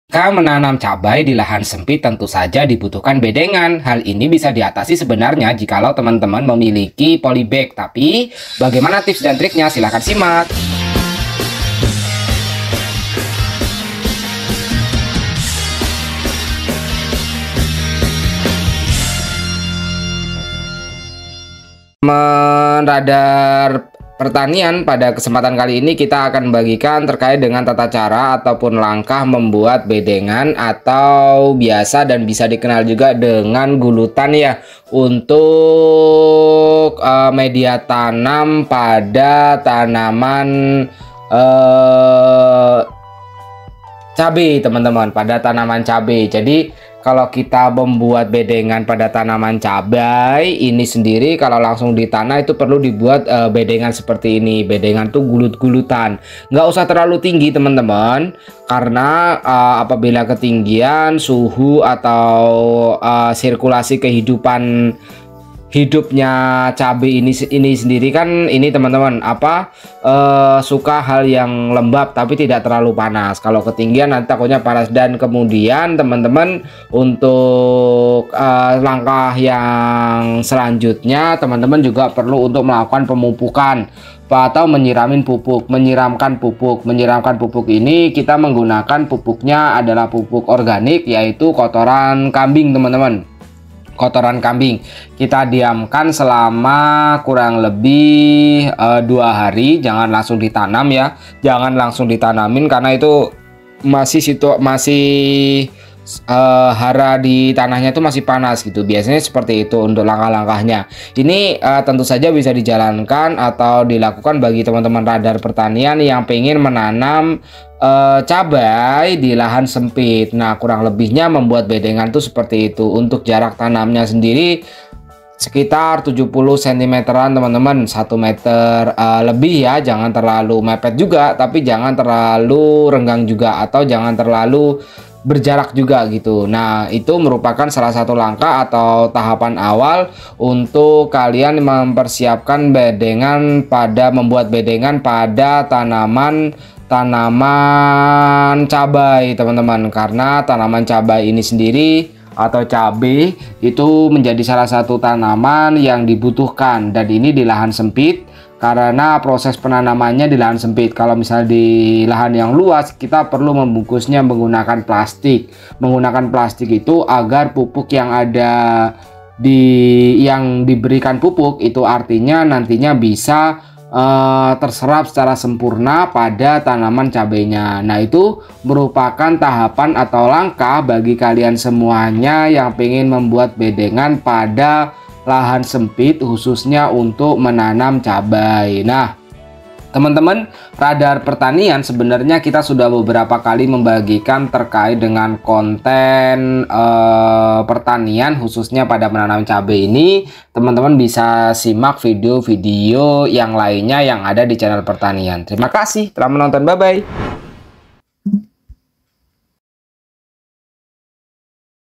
Cara Bikin Bedengan Cabai yang Tepat
Cara bikin bedengan cabai merupakan bagian dari upaya petani agar menjadikan tanaman cabe lebih subur dan terawat yang tentunya diharapkan akan dapat menghasilkan buah yang lebih banyak dengan kualitas yang bagus.

Pengolahan tanah untuk cabe bisa diterapkan sistem bedengan mulsa ataupun bedengan tanpa mulsa. Pada kesempatan kali ini @RadarPertanian akan membagikan cara bikin bedengan cabai tanpa mulsa agar menghasilkan buah yang maksimal.